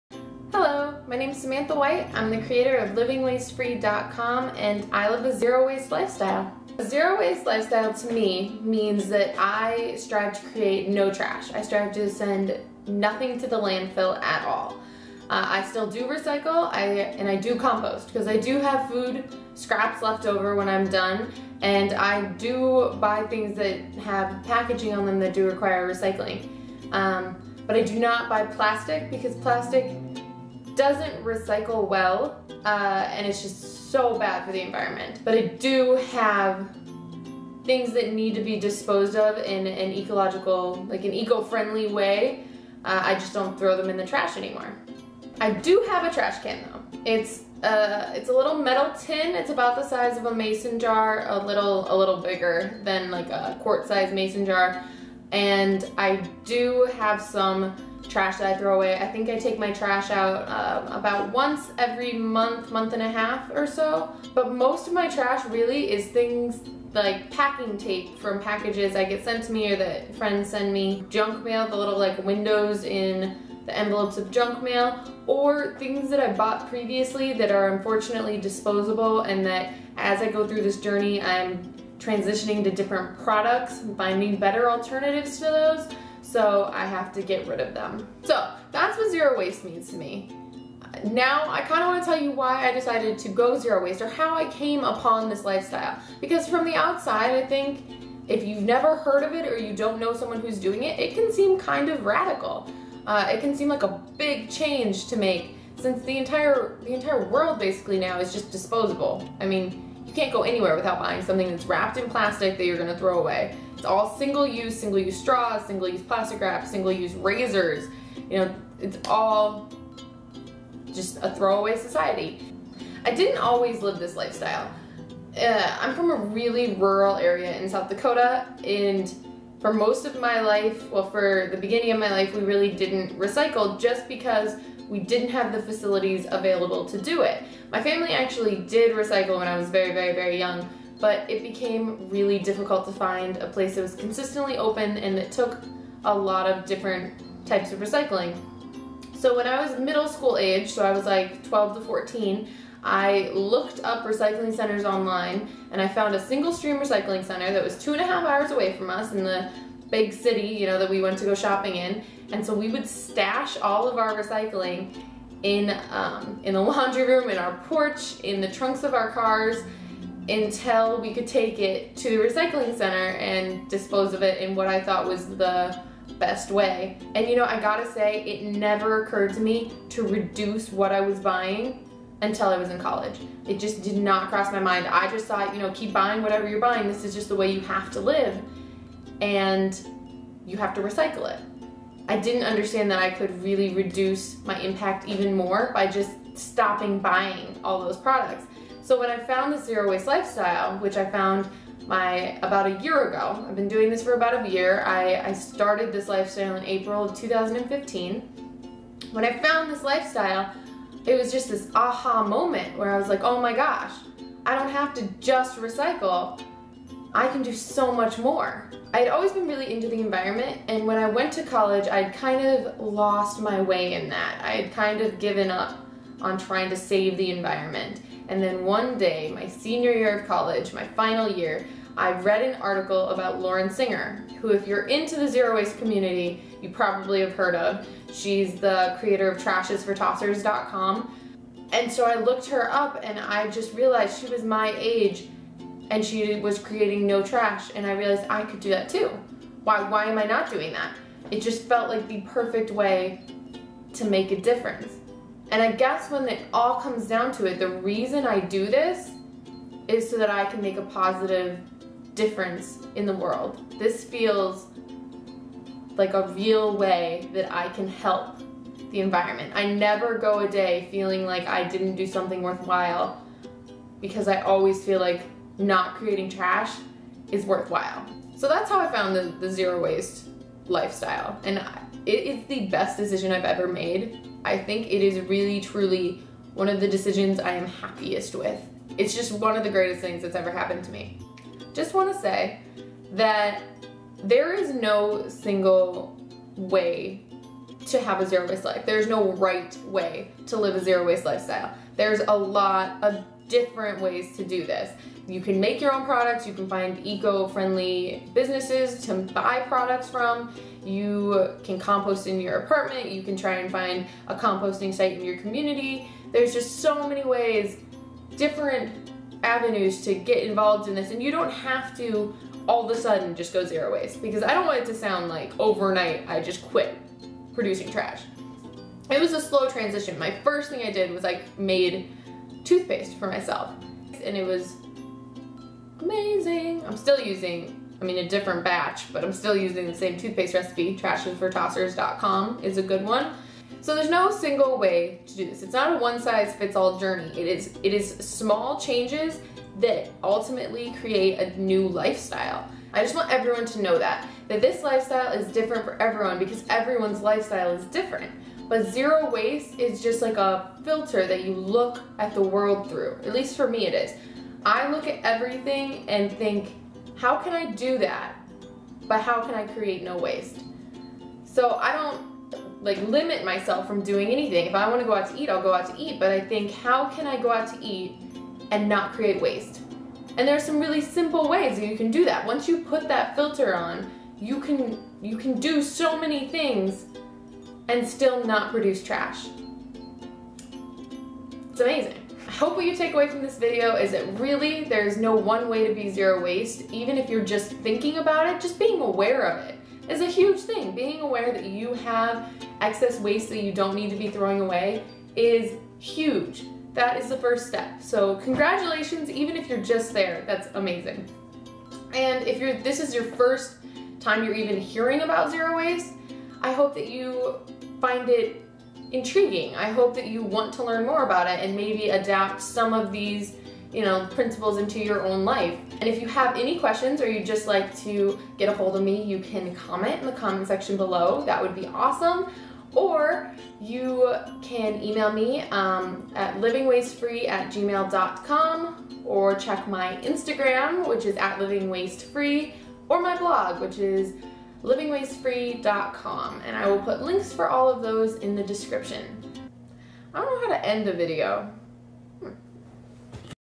Why I Went Zero Waste
I'm talking all about what zero waste means to me and how I got into this lifestyle. I hope you enjoy!

Wanna check out the recipe that got me started?

The music for this video was created by the awesomely talented Jameson White